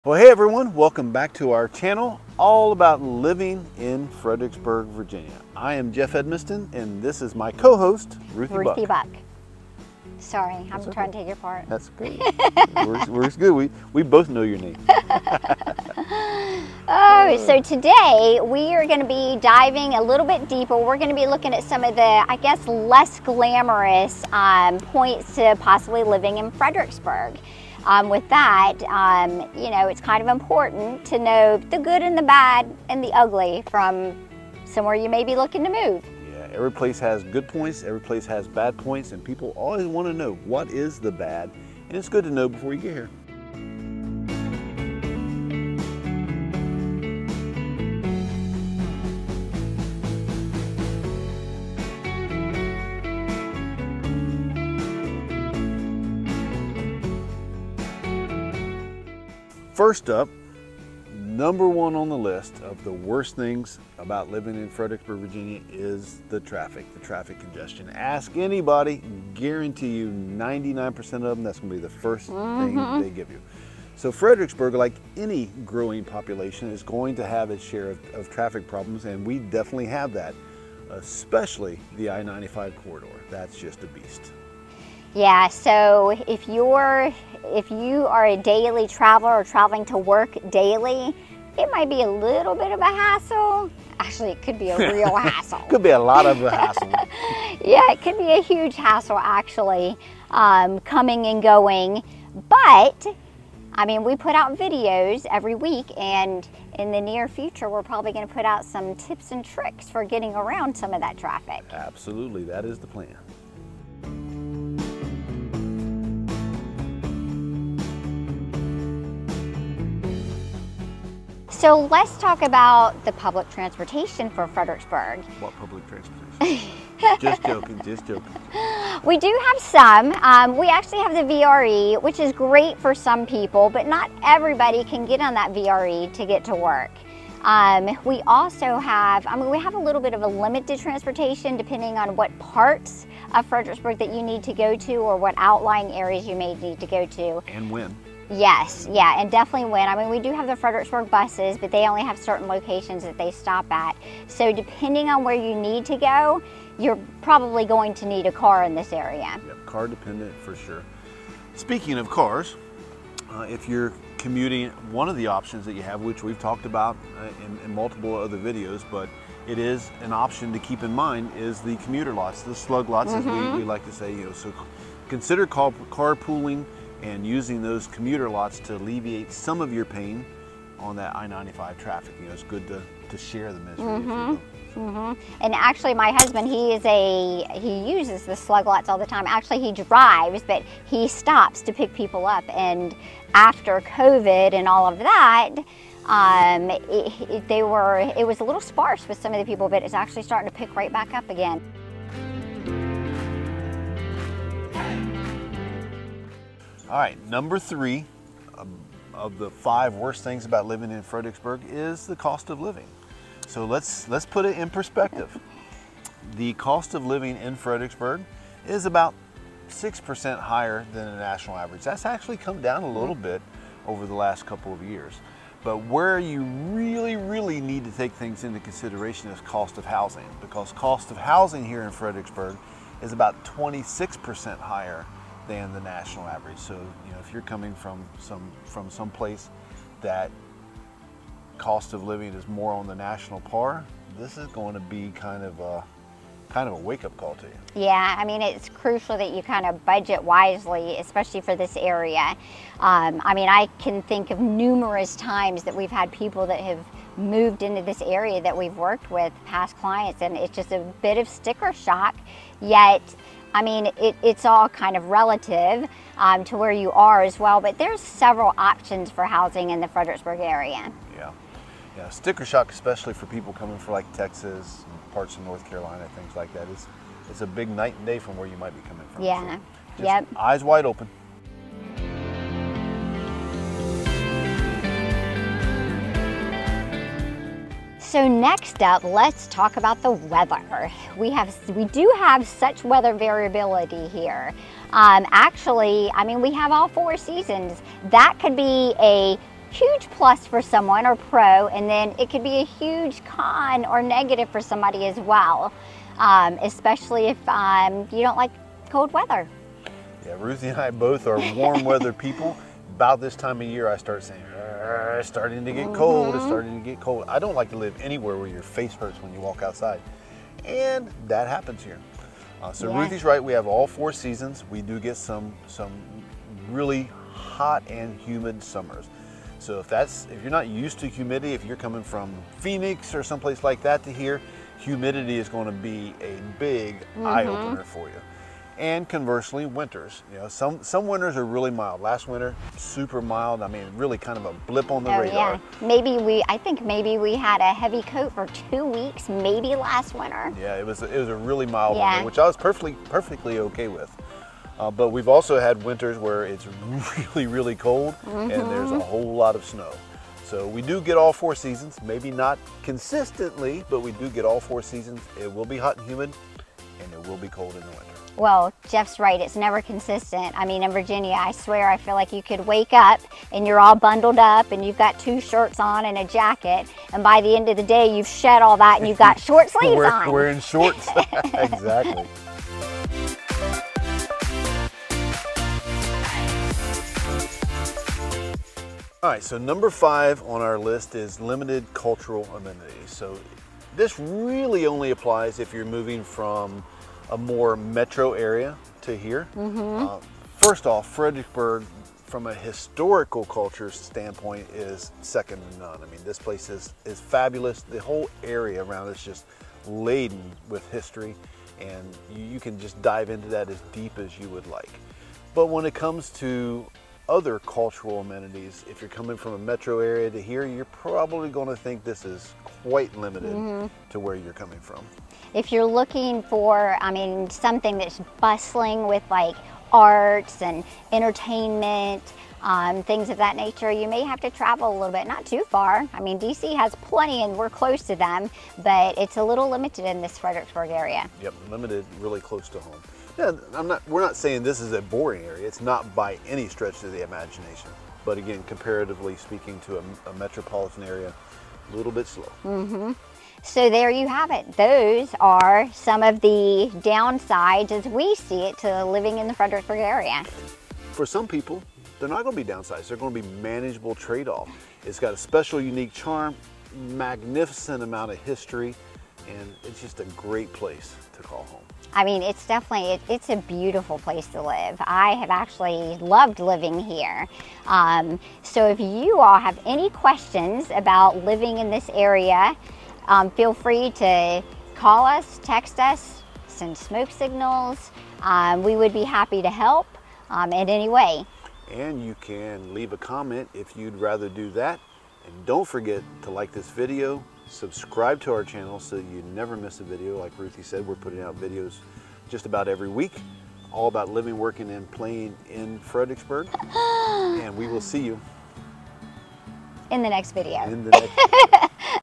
5 Worst Things About Living in Fredericksburg, VA
5 Worst Things About Living in Fredericksburg, VA

In our latest video, hosts Jeff Edmisten and Ruthie Buck take you on a journey through the less glamorous side of living in Fredericksburg, VA. As much as we adore this beautiful city, it's crucial to consider all aspects before making the move.

Topics Covered:

Traffic Congestion: We dive into the traffic situation in Fredericksburg, especially along the infamous Interstate 95 during rush hours. Don't worry; we've got tips to help you navigate it effectively.

Limited Public Transportation: Explore the public transportation options in Fredericksburg, from the VRE commuter train to carpooling and ridesharing solutions.

Cost of Living: Housing costs have been on the rise in Fredericksburg, and we'll discuss how to budget accordingly when relocating to this vibrant city.

Weather Variability: Discover the four seasons of Fredericksburg, from cold winters to hot and humid summers, and how it might affect your lifestyle.

Limited Cultural Amenities: While the city boasts a rich historical heritage, we'll address the available cultural amenities, which may not be as extensive as in larger cities.

Join us as we provide insights into the pros and cons of life in Fredericksburg, Virginia. Remember, what might be a downside for some can be a manageable tradeoff for others. Fredericksburg offers its unique charm, historical significance, and natural beauty that make it a fantastic place to live.

We're here to assist you in making informed decisions about life in Fredericksburg.

Jeff Edmisten & Ruthie Buck
Samson Properties
1440 Central Park Blvd, Suite 210
Fredericksburg, VA 22401

Licensed in the Commonwealth of Virginia